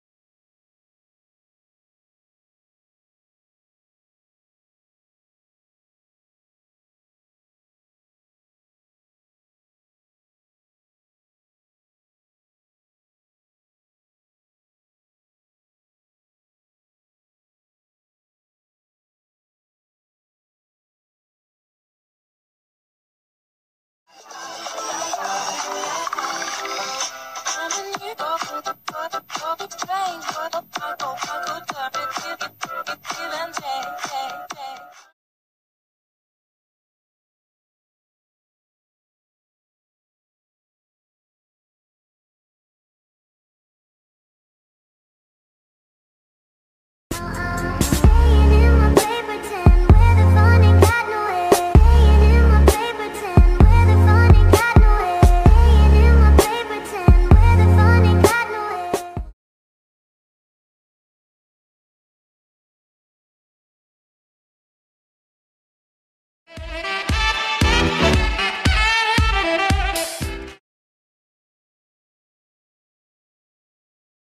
(Some) of my friend animators then vs now
Videos I used and link:

Cupcake animations

Circlecats animations

Itty bitty moon/snowberry

Thanks for all the support and being my friends! (Ps I may make a sequel to this) (pss I made this maybe a month ago so if those animators have better videos to put in the “now” category I don’t wanna hear about it)